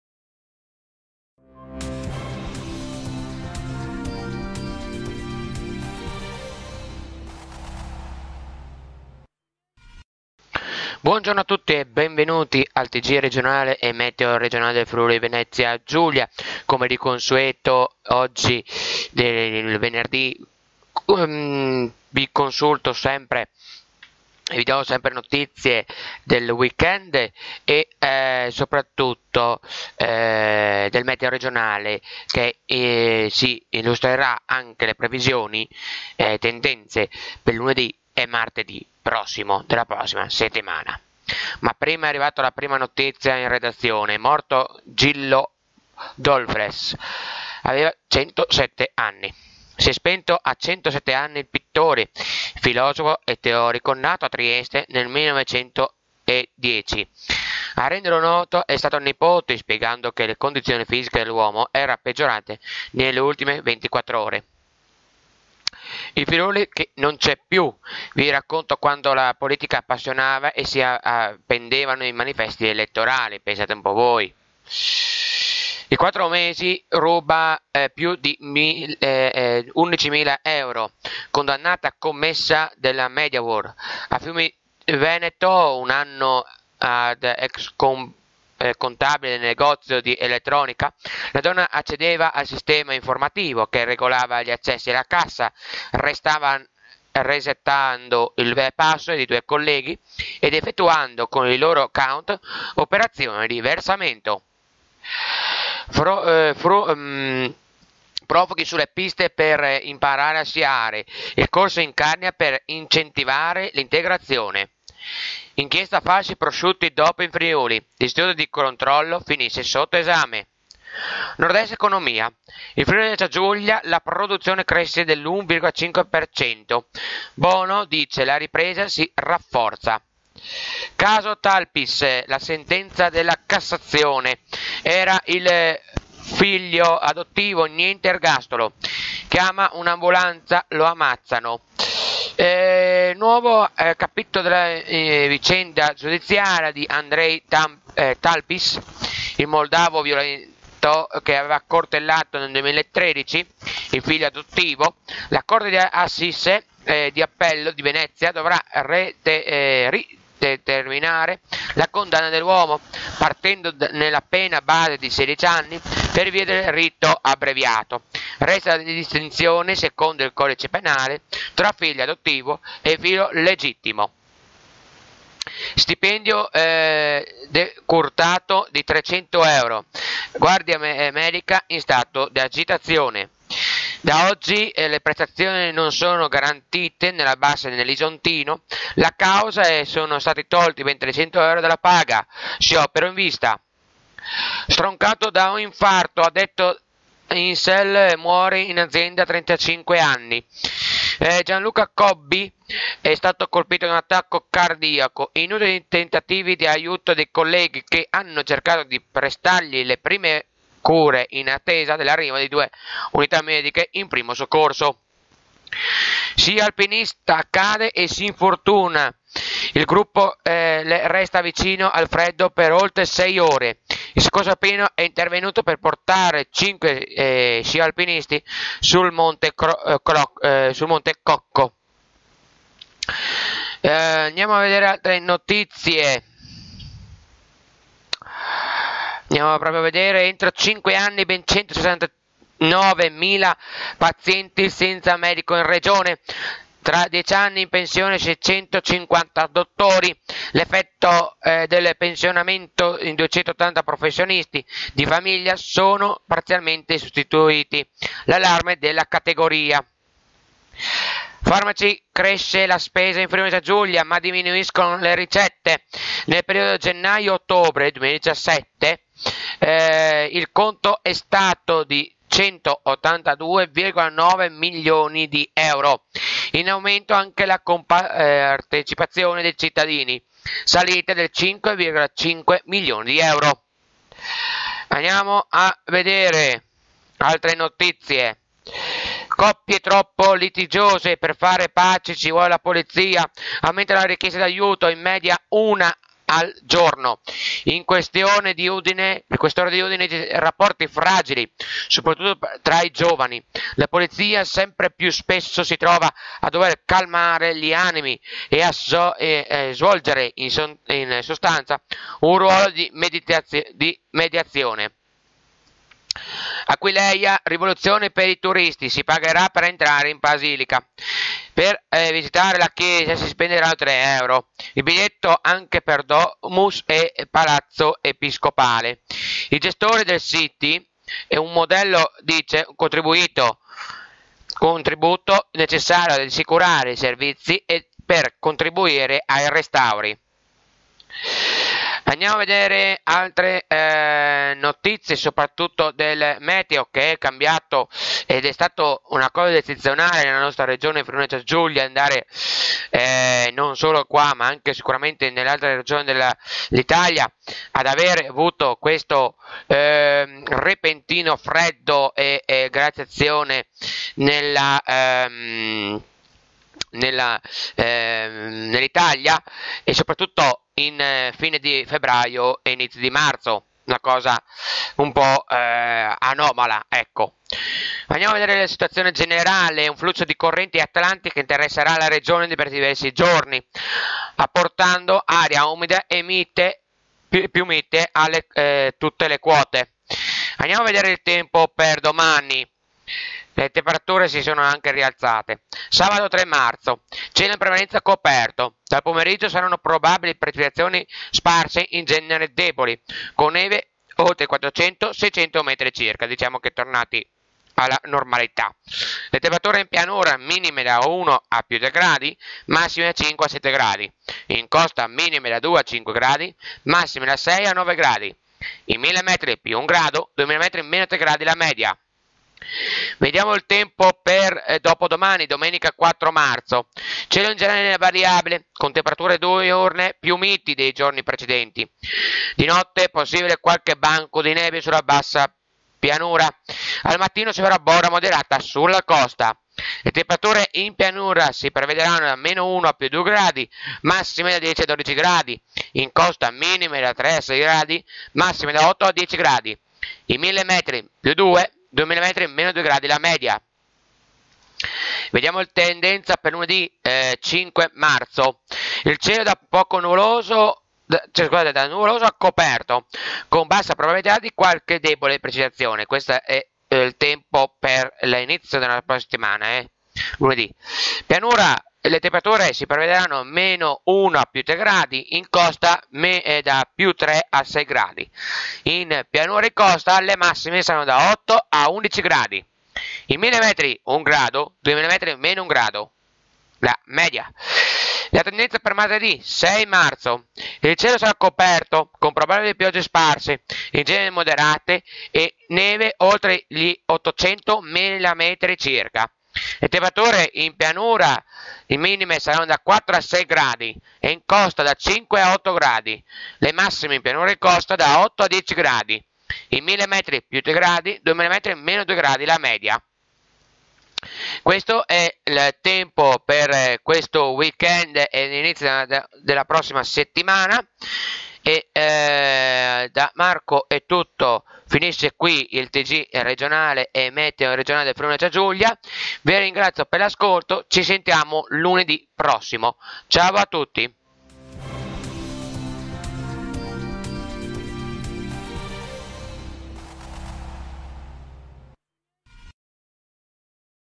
QUI UDINE: Marco- TG News e Meteo FVG 2/3/2018
Notizie regionali e meteo FVG di Audio Radio FVG NordEst, diretto da Marco Puzzolo di Audio Radio FVG NordEst, Rdaradio1 e Stapradio. Consultate sempre le previsioni Meteo su Internet e sulle App.